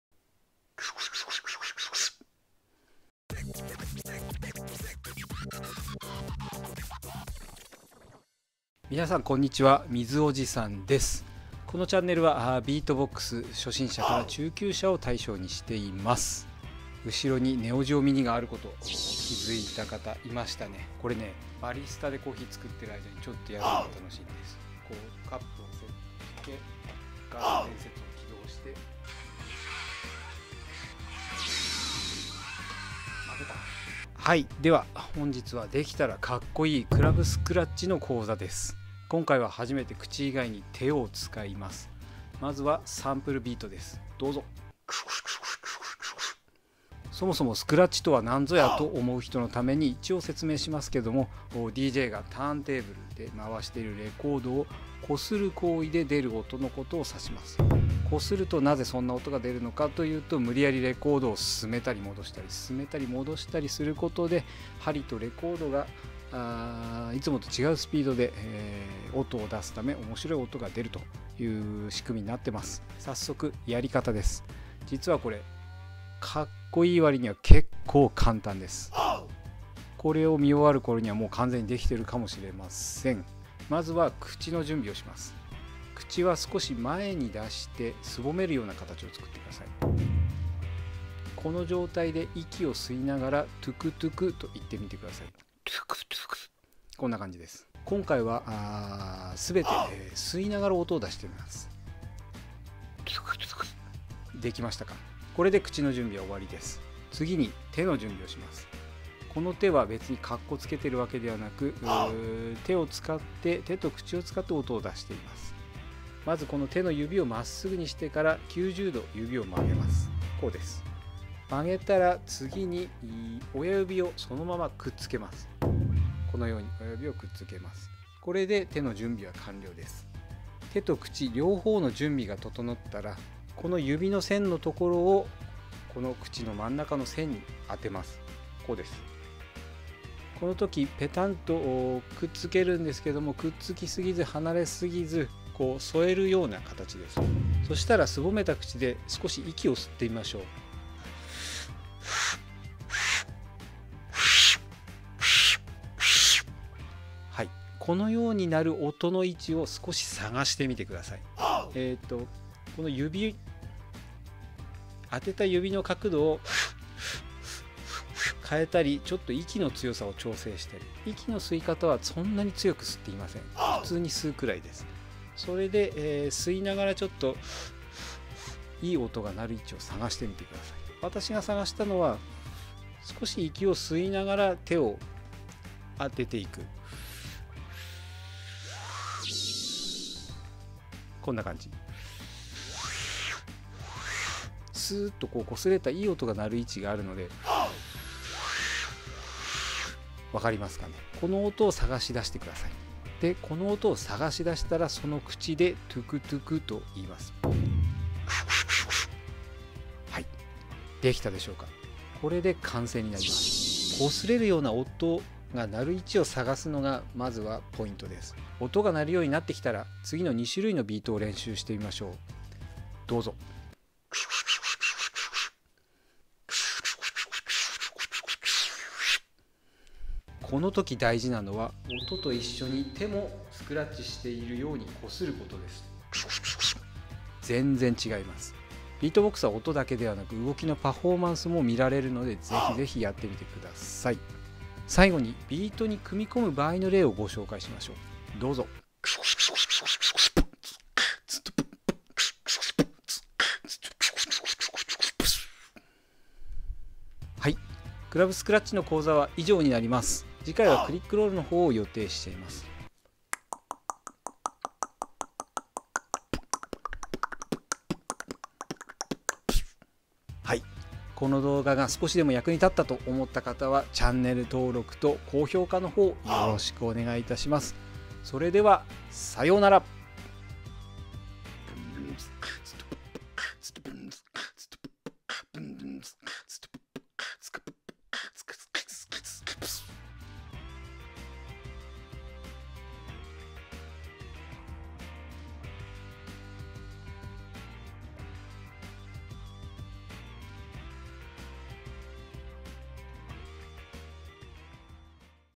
【ビートボックス講座(ボイパ講座)】#6 クラブスクラッチ
40代おじさんのみずおじさんがビートボックスを教えます。
対象は初心者から中級者になります。

質問や不明点、この辺がわからない等はコメントください！